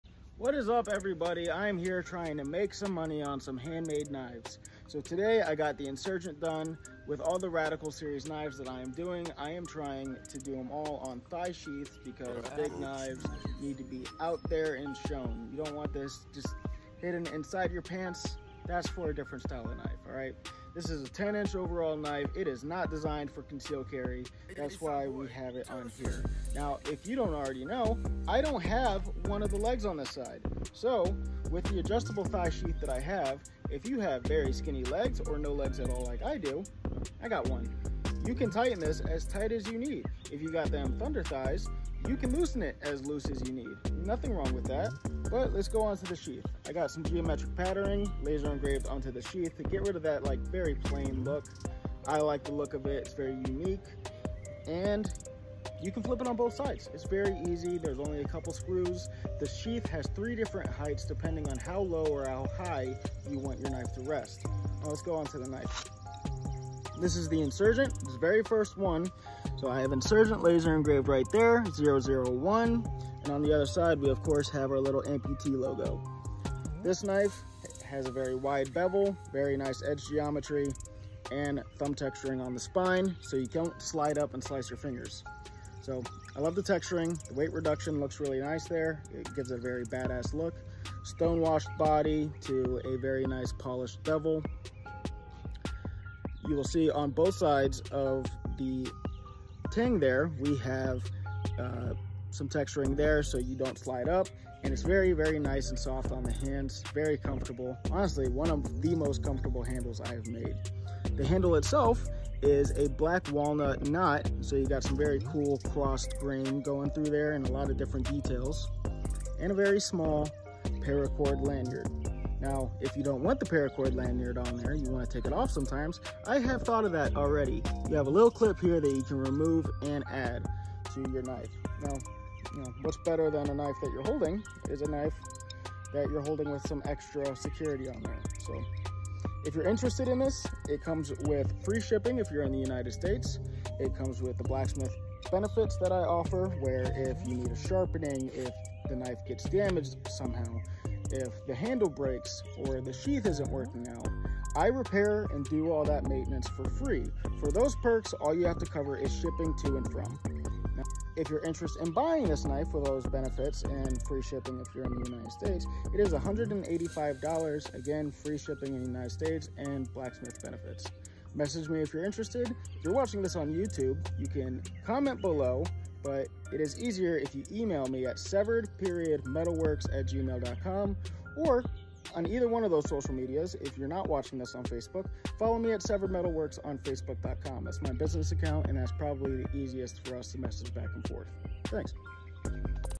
Insurgent #1 is done and ready for purchase!$185 free shipping in US!
Insurgent #1 is done and ready for purchase!
$185 free shipping in US
Steel type: 8670 High Carbon
Handle: Black Walnut
Custom kydex sheath
Overall length is 10 inches